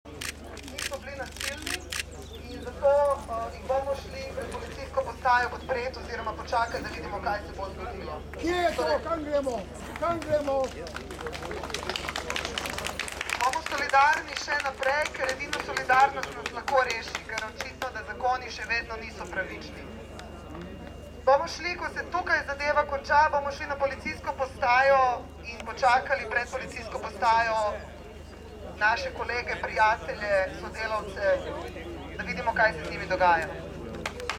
Policijski pes za iskanje eksploziva
Ljubljana/Litija, 16. marca (STA) - Policija je ob današnji deložaciji družine Vaskrsić v Litiji doslej pridržala 19 oseb, ena je bila pri tem lažje telesno poškodovana. Kot so pojasnili na ljubljanski policijski upravi, so policisti ukrepali v skladu s pooblastili, uporabili pa so "najmilejša prisilna sredstva".